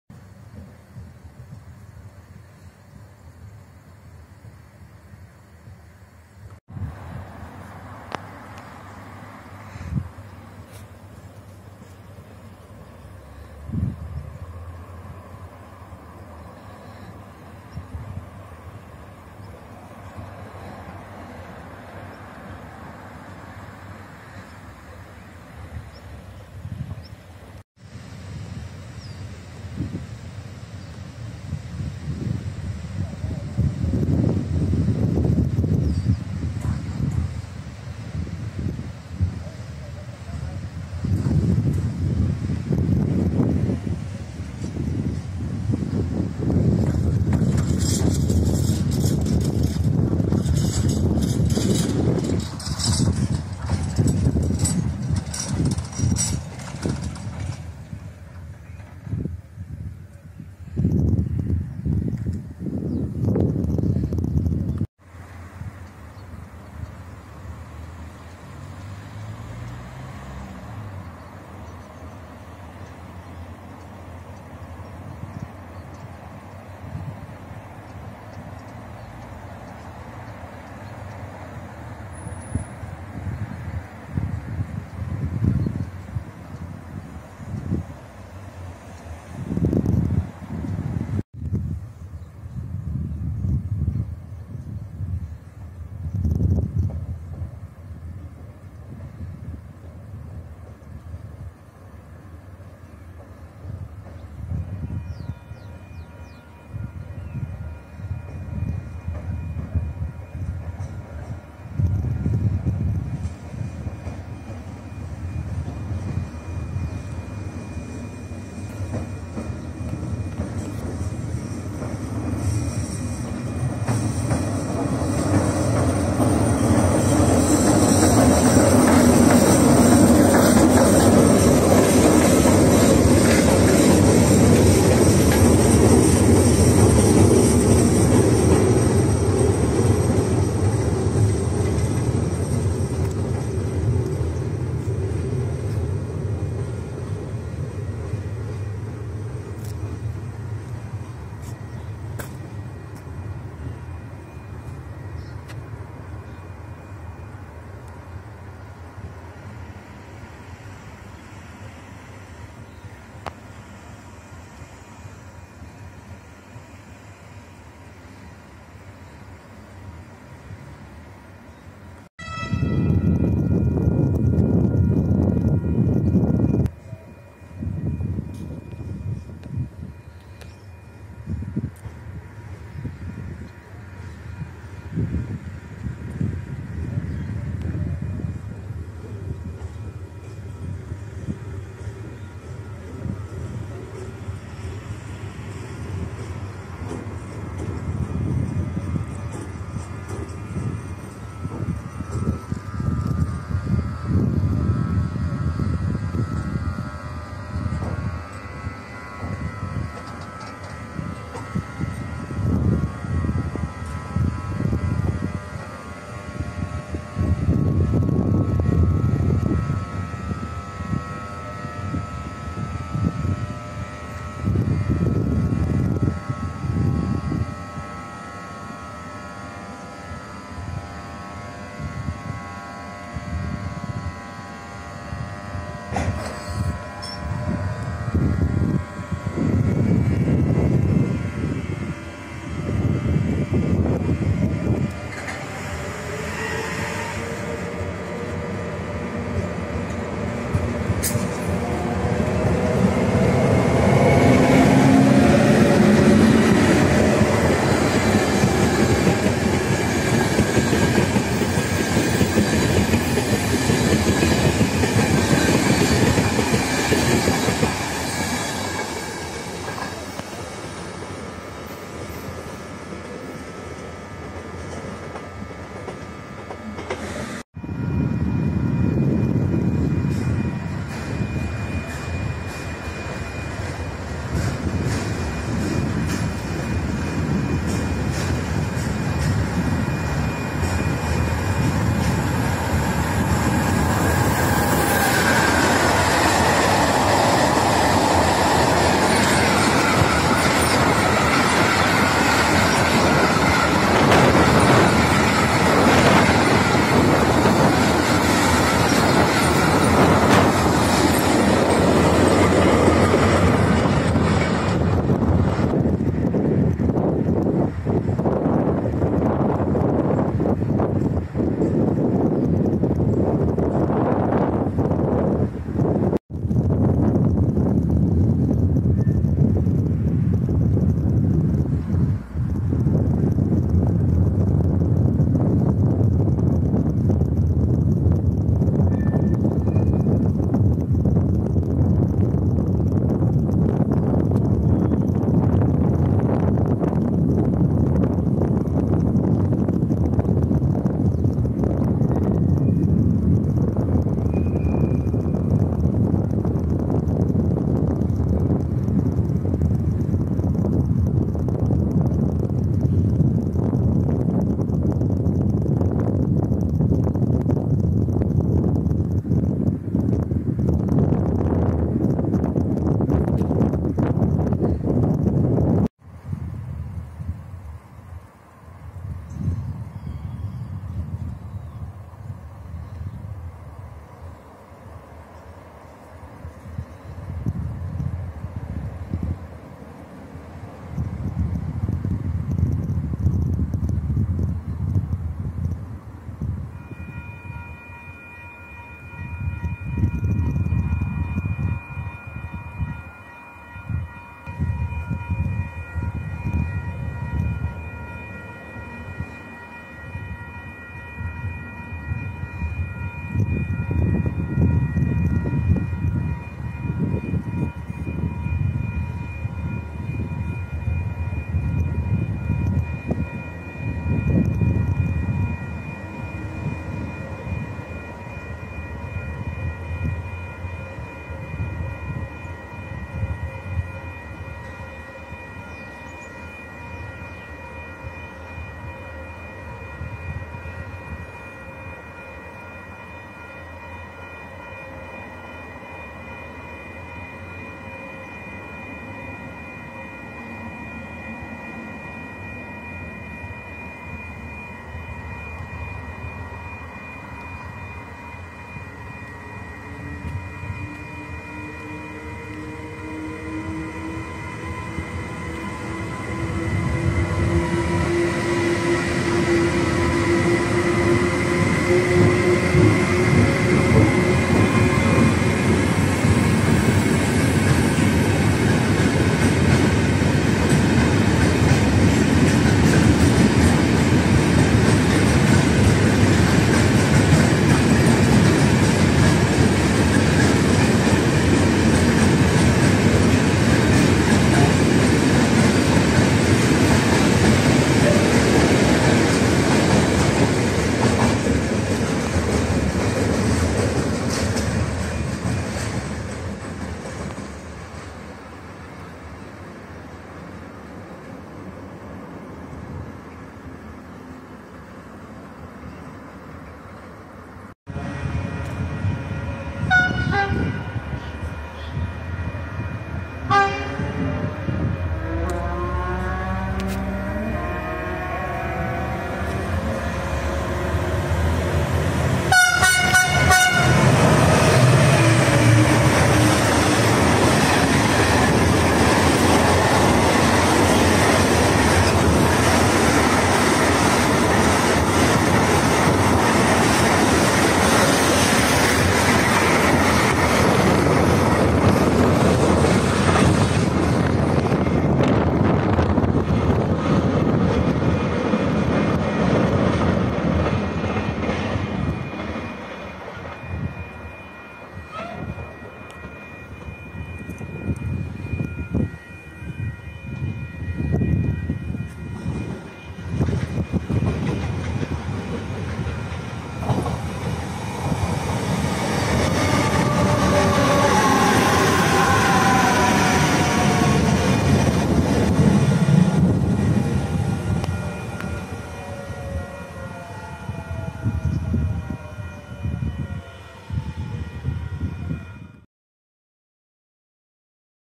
Comboios em Alfarelos - 11dec21 - 5ª Parte
Movimentação de alguns comboios nesta estação da linha do Norte com ligação ao Oeste